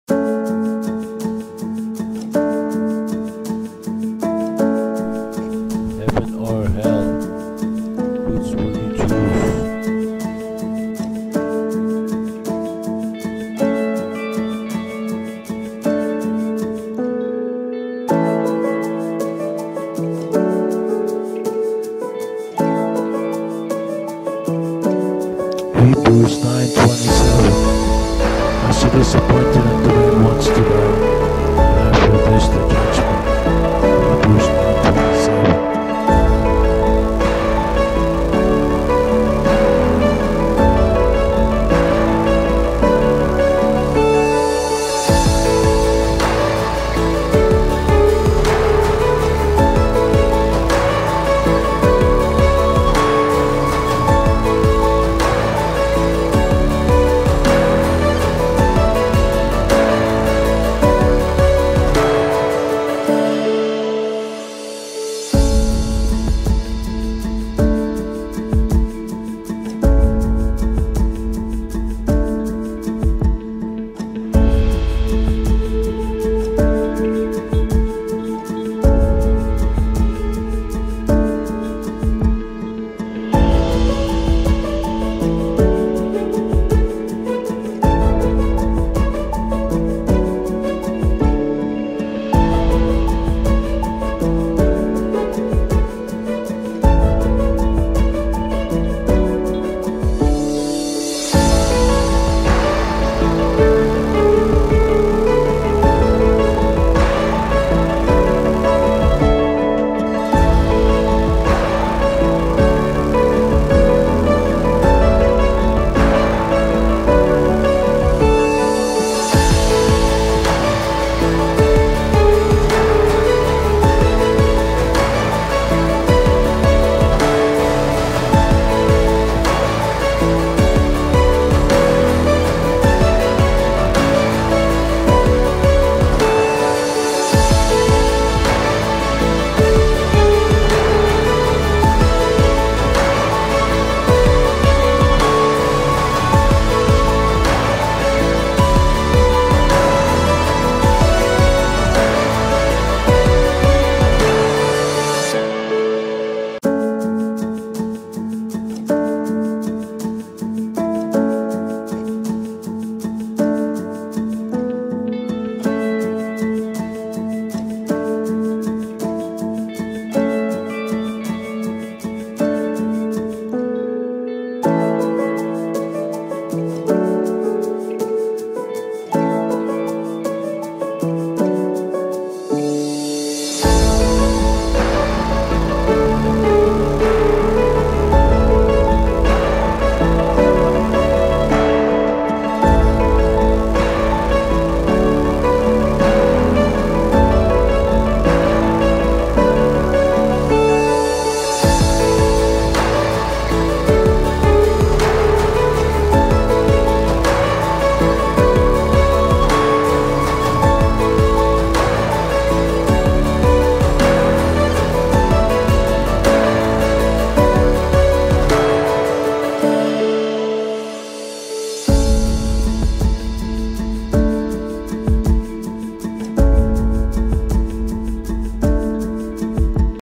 Finally Completed This Latest Gospel Video Project (12/10/23)
So, yours truly finally completed this my latest gospel video project (12/10/2023). It is based on #115 tract of Fellowship Tract League, titled "Heaven Or Hell: Which Would You Choose?"